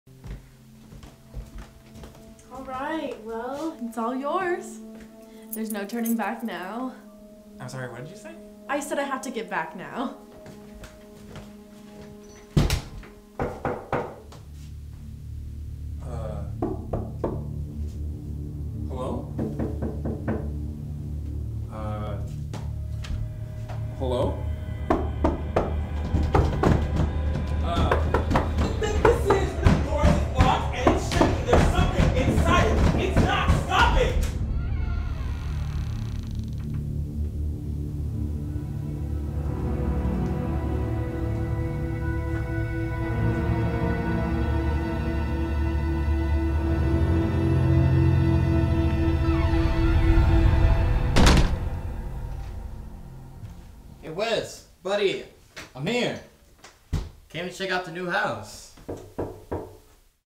Your New Home (2020) | 1 Minute Film Challenge
Welcome to your new home. Our third submission for Film Riot's One Minute Short Film Competition.

Story by
CORBAN GILILLAND

Starring
TRE MCLEOD
JESSICA HARMS
LJ BRODIE

Directed by
RANKIN DEAN
BRODIE JASSO

Cinematography & Editing by
RANKIN DEAN